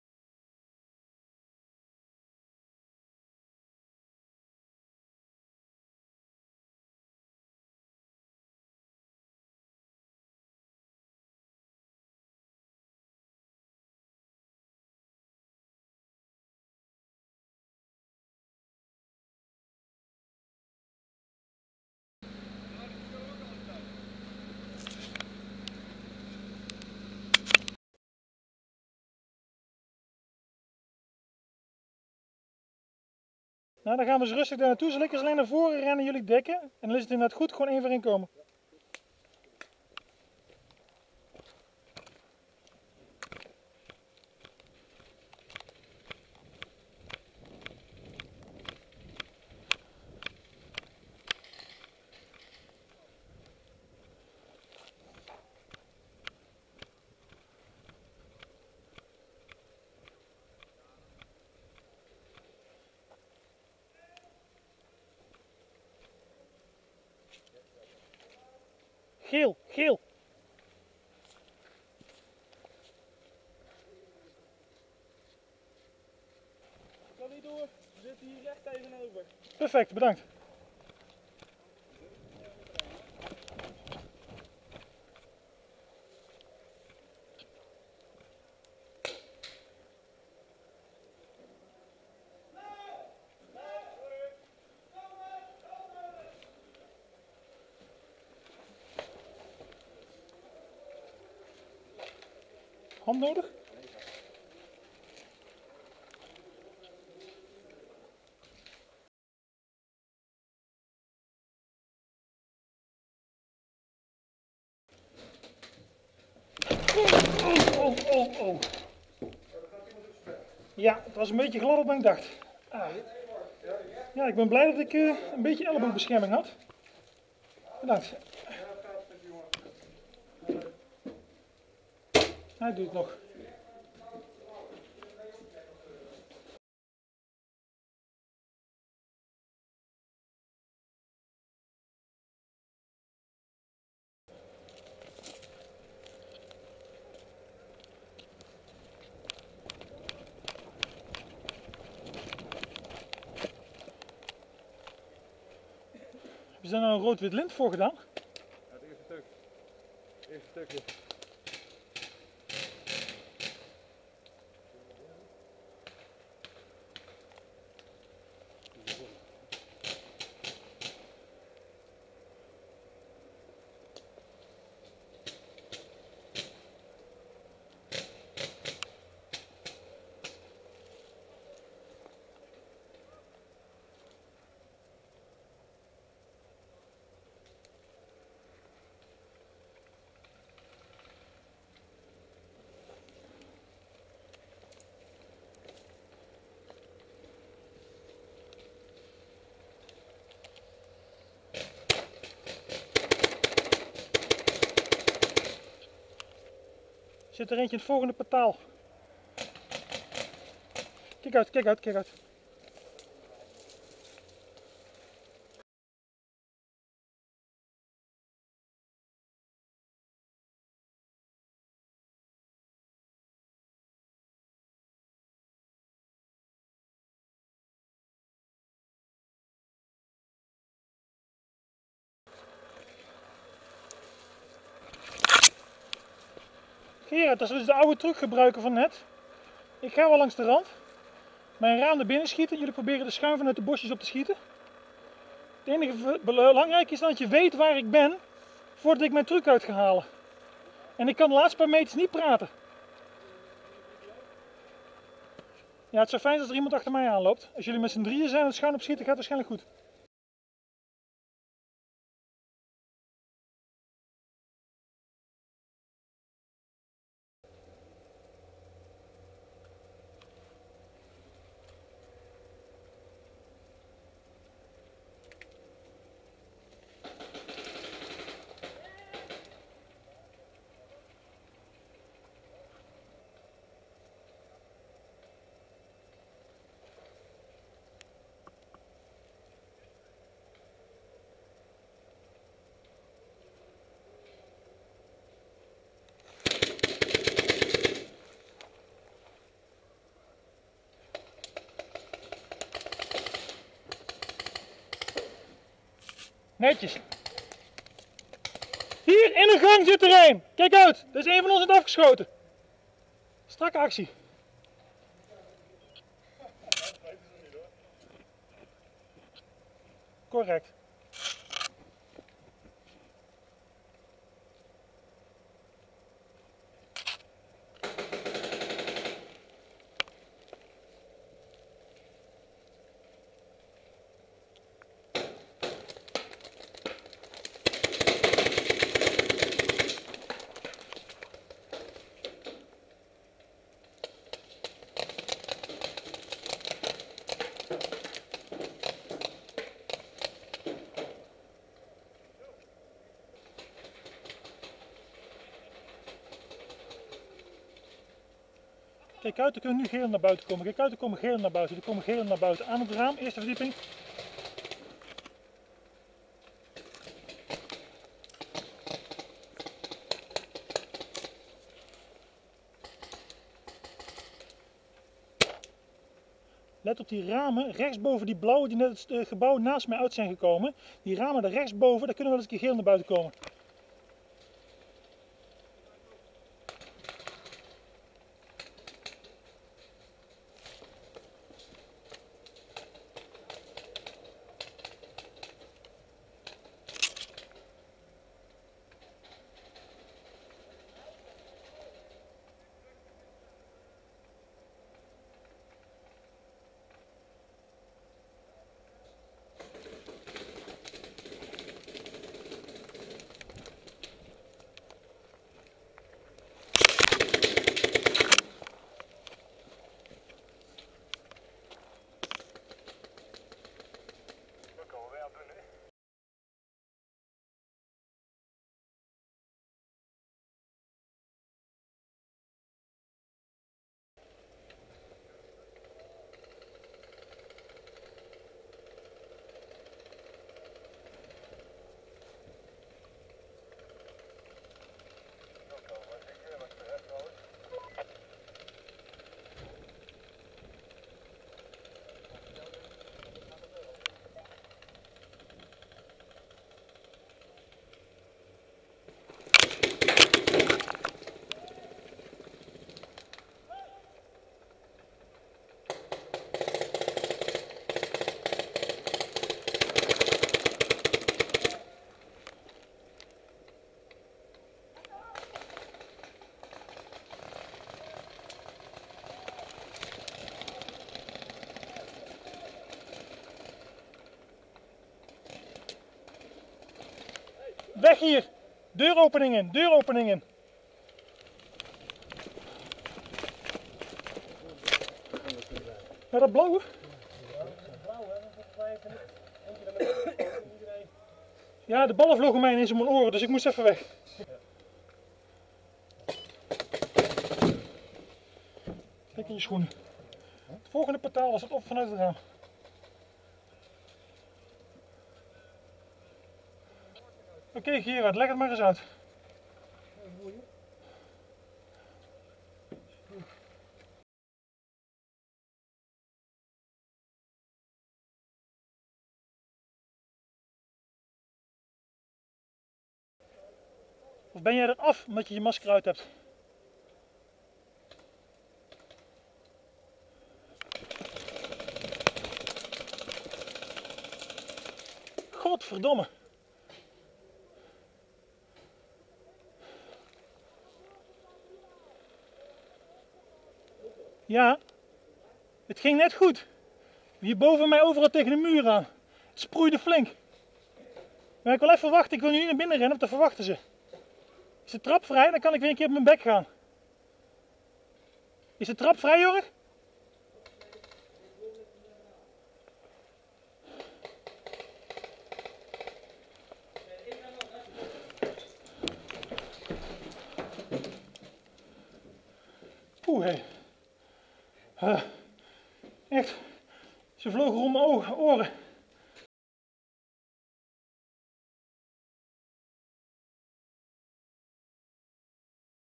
Paintball FPS Mahlwinkel 19-09-2009, part 1
Compilation of some of the action on our second day (out of three) of the September 2009 Mahlwinkel Big Game in Germany. I added a map of the field, so that it's possible to see where the scenes are shot. The talking I do is usually over the radio with my buddy Gerard, or with other players in my vicinity. So no, I'm not talking to myself! :-) I have not taken the trouble to subtitle it, since translating all the Dutch, German and English I use would take up a lot of time that I'd rather spend on converting the remainder of my HD video clips from Mahlwinkel.

There is a blooper in this clip, when I came bouncing down the slippery stairs, just after the opponents have plastered it with paint... Good thing i was wearing elbow guards! :-)